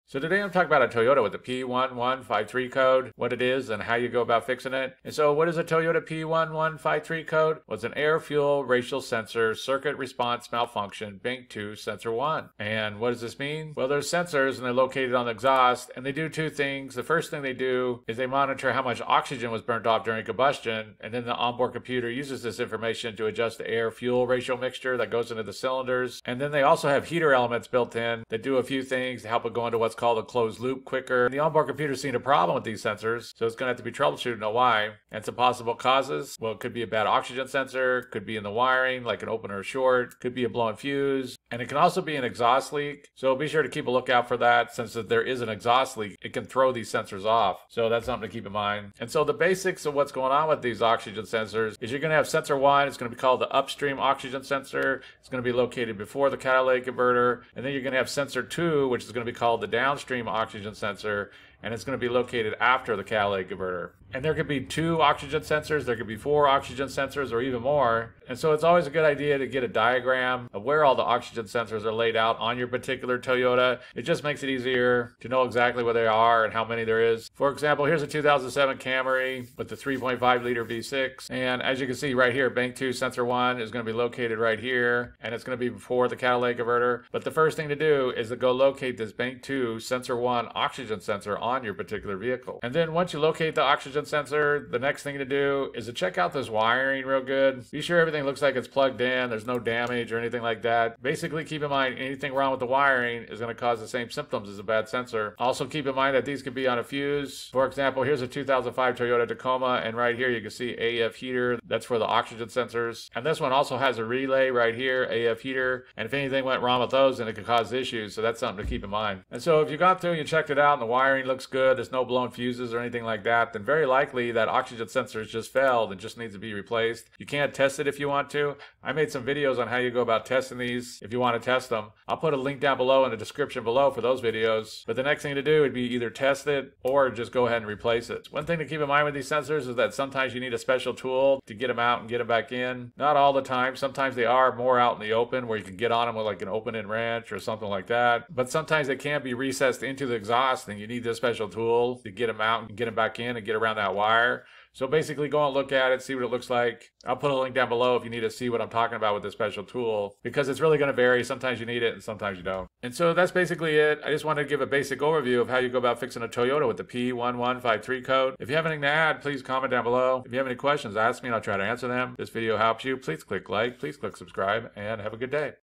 How to Fix Toyota P1153 Code: Air/Fuel Ratio Sensor Circuit Response Malfunction Bank 2 Sensor 1
Causes and Fixes Toyota P1153 Code: Air/Fuel Ratio Sensor Circuit Response Malfunction Bank 2 Sensor 1
O2 Sensor removal tool on Amazon

Possible Causes of a Toyota P1153 Code:
Bad A/F Heater Oxygen Sensor
Wiring Open or Short
Blown Fuse
Exhaust Leak